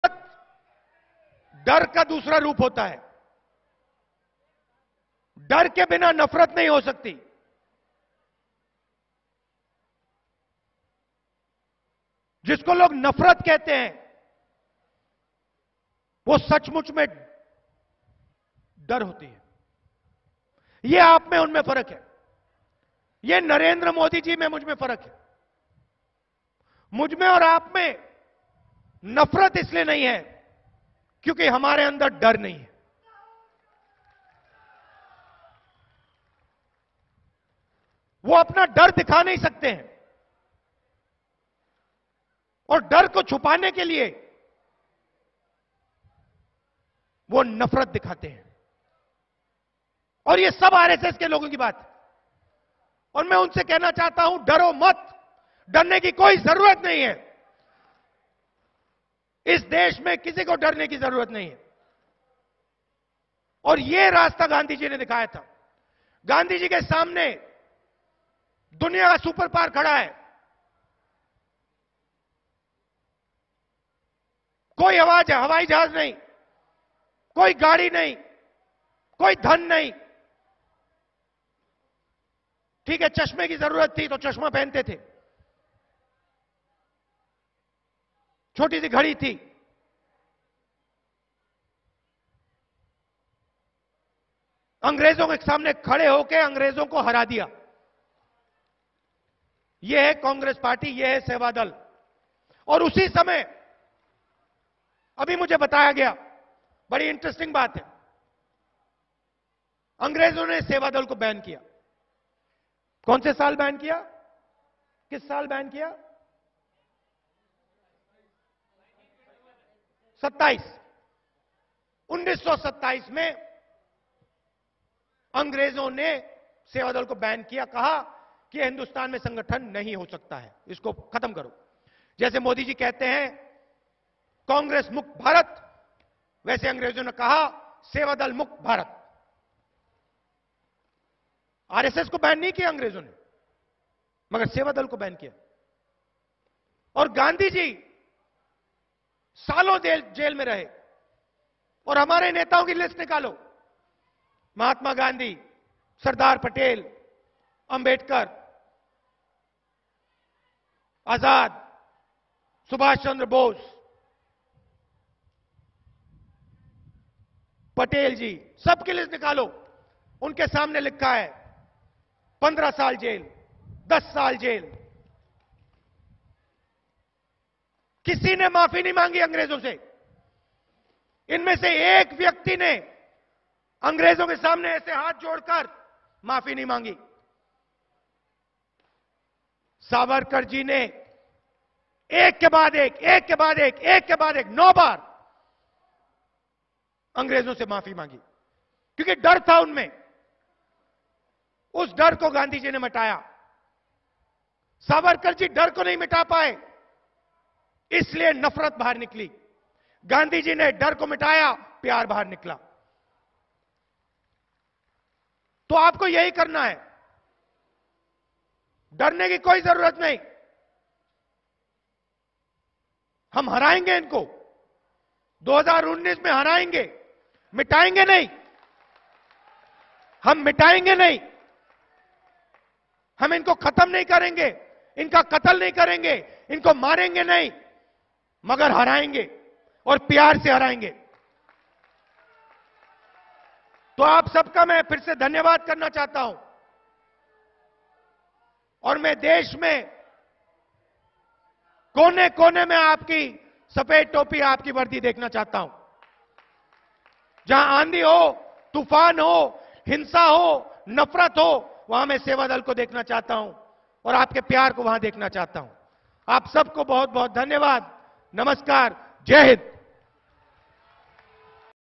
HCN News | राहुल गांधी ने सावरकर को दे डाली गाली, मच गया बवाल | Rahul Gandhi Speech Today
कांग्रेस अध्यक्ष राहुल गांधी मिशन 2019 पर गुरुवार को राजस्थान के अजमेर में थे. अजमेर में उन्होंने कांग्रेस सेवा दल के कार्यक्रम को संबोधित किया. राजस्थान के मुख्यमंत्री अशोक गहलोत और डिप्टी सीएम सचिन पायलट भी इस मौके पर मौजूद थे. राहुल गांधी ने अजमेर में कांग्रेस सेवादल के दो दिवसीय राष्ट्रीय महाअधिवेशन को संबोधित किया.